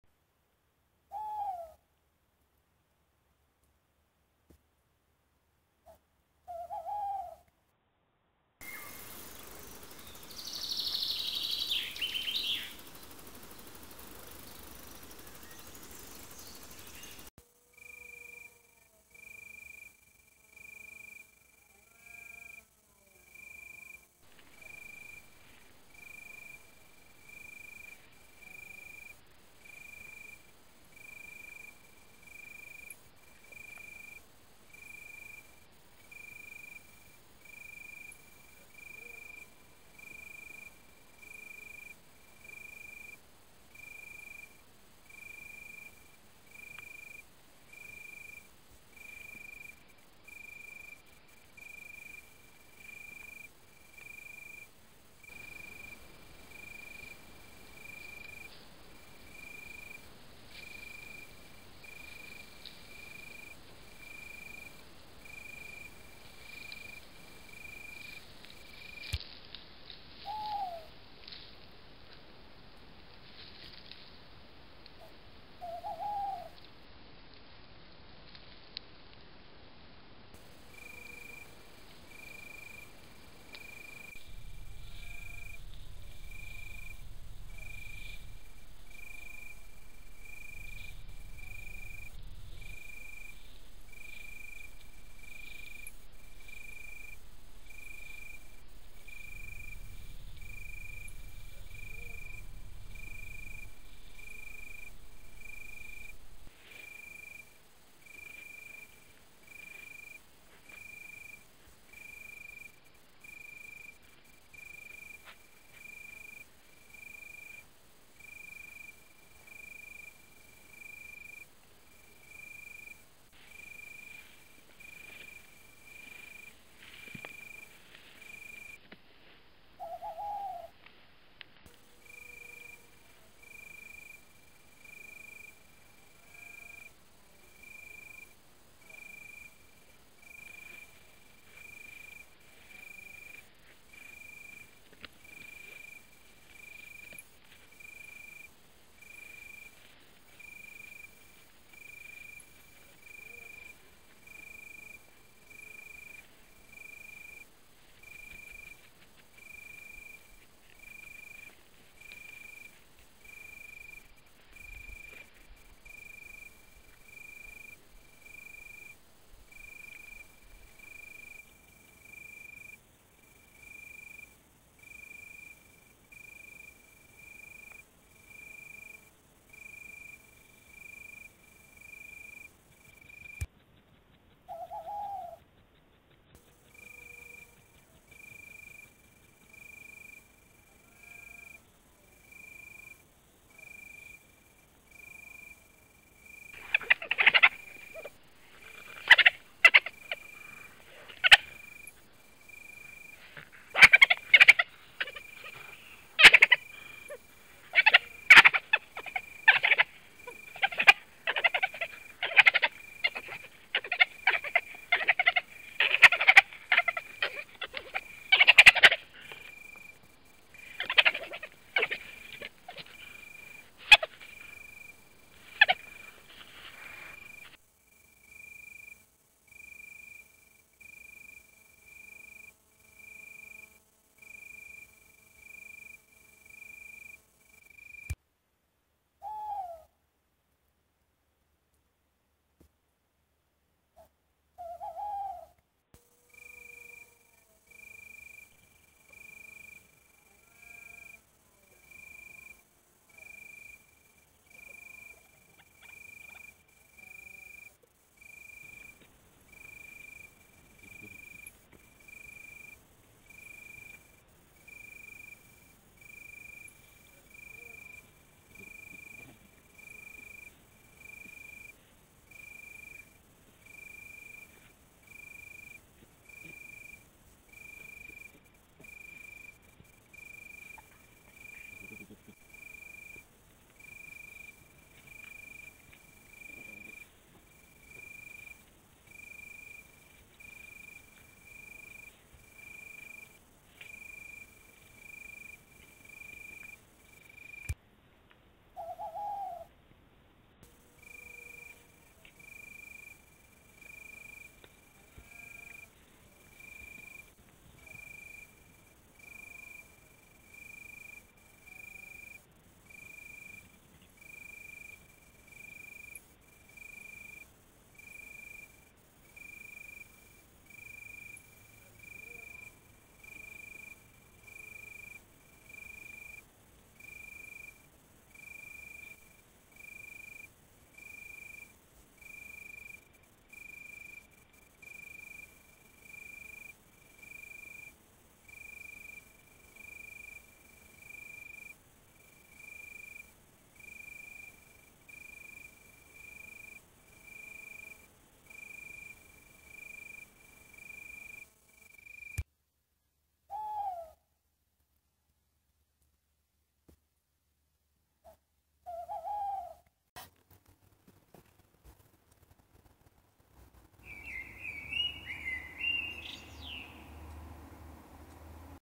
El Tejón , Meles meles
Secuencias de vídeo realizado mediante fototrampeo a varios tejones